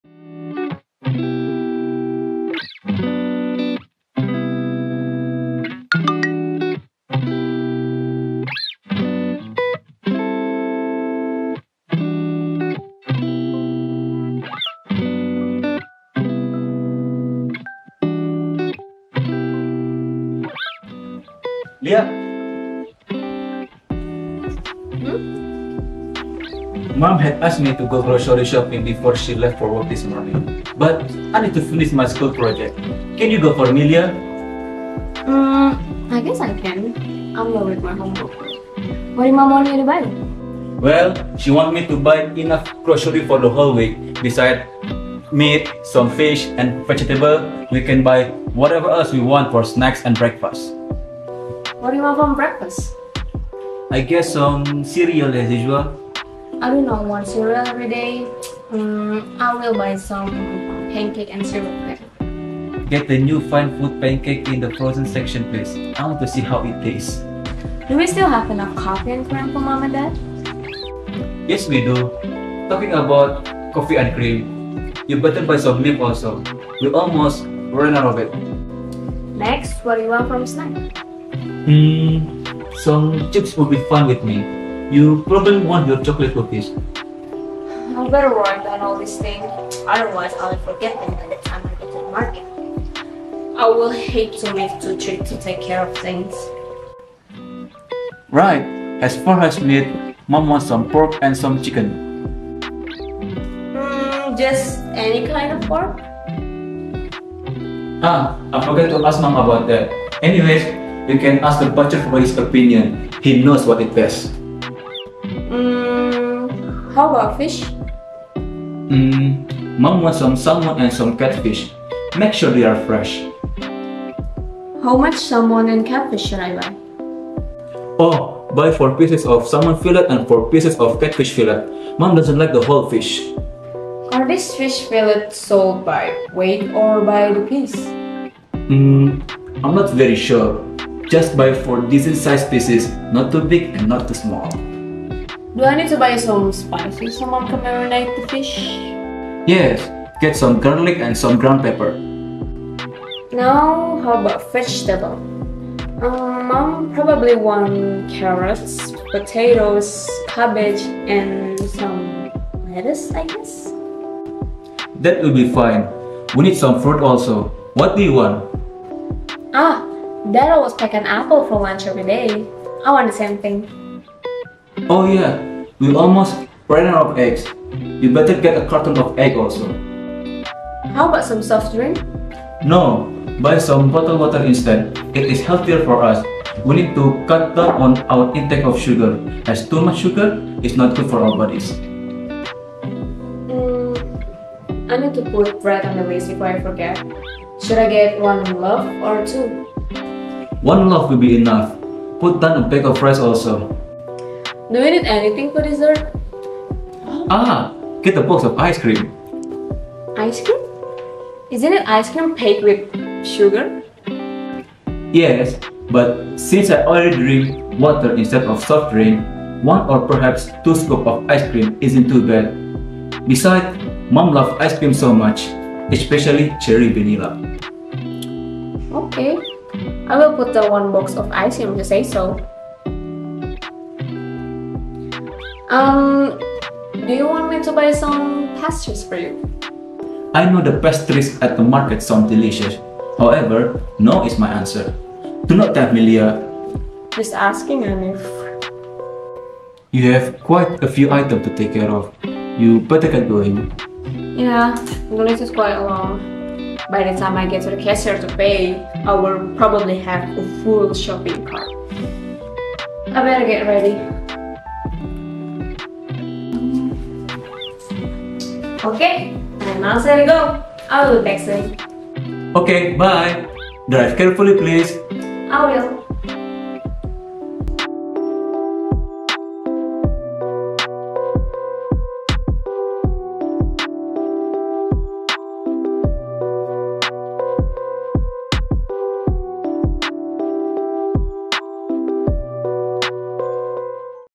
Going Shopping at the Market
It is an excellent video simulation within the context of Going Shopping. Gina and Hanif from the Class of 2022 performed well in the conversation. This is the final test video for the class Listening and Speaking for Contextual Situations under the supervision of Tatan Tawami, M.Hum. Please enjoy.